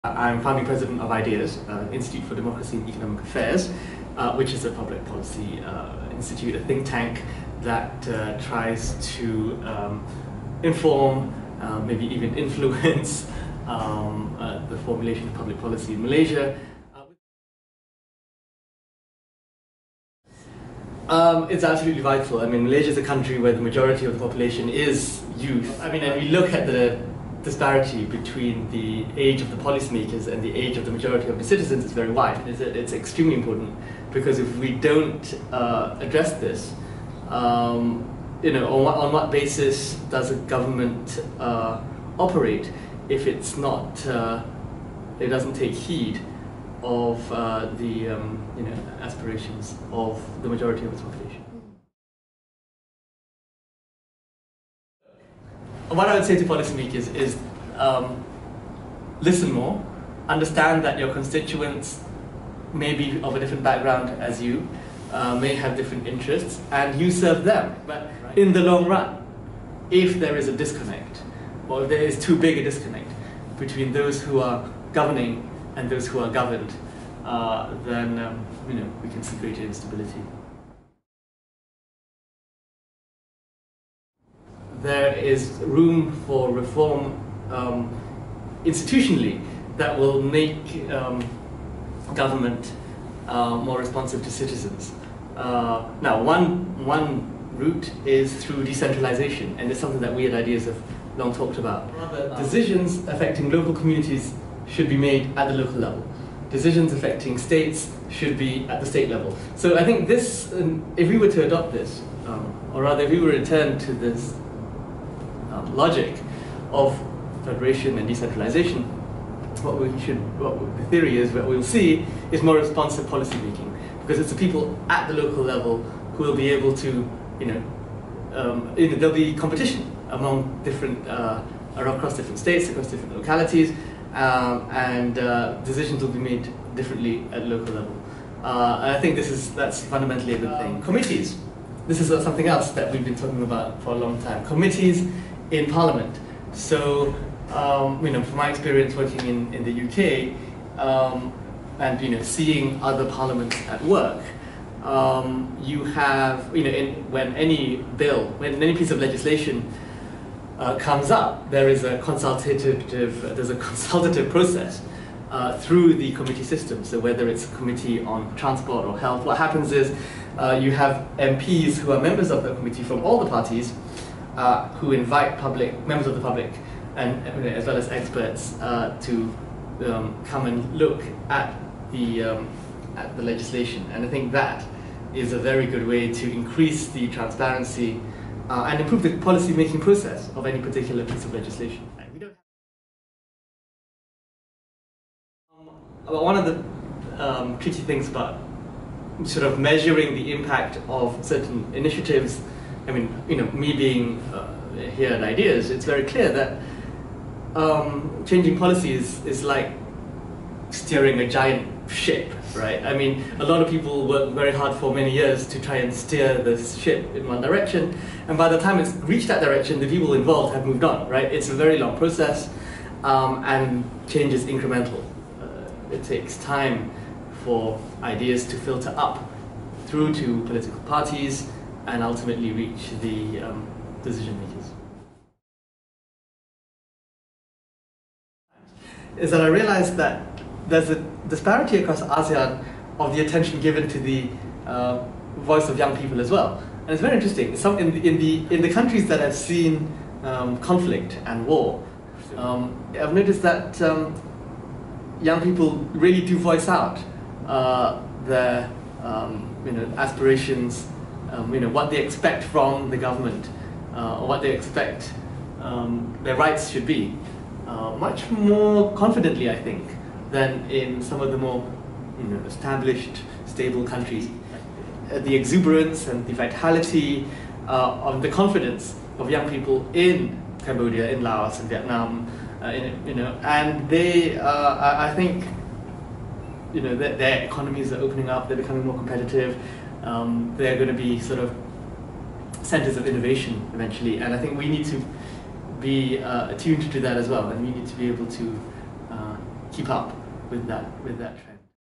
What should policymakers know? - Tunku Zain Al-'Abidin of IDEAS
Tunku Zain Al-'Abidin of the Institute of Democracy and Economic Affairs discusses what policymakers should know about engaging youth.